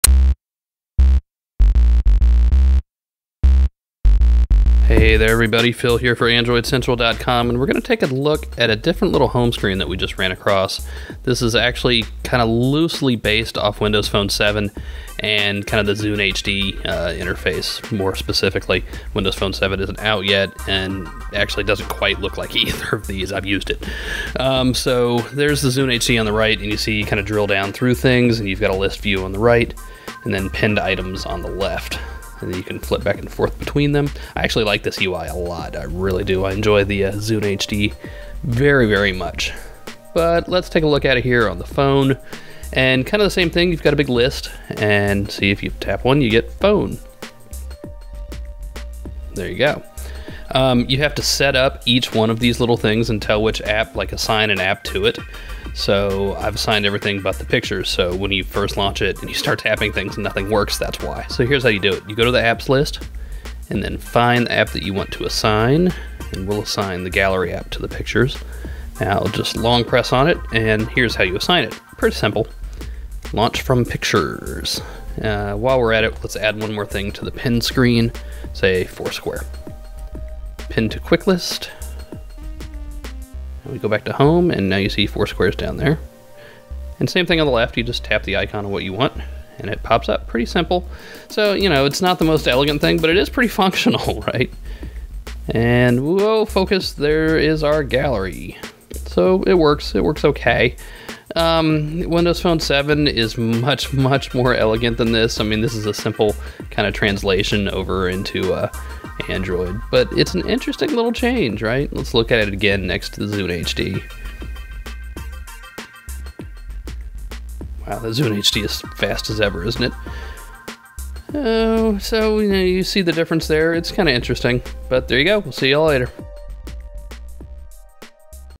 Zune Home Android launcher
Phil takes a look at the Zune Home Android launcher replacement, making your phone look a bit like the Zune and Windows Phone 7 UIs.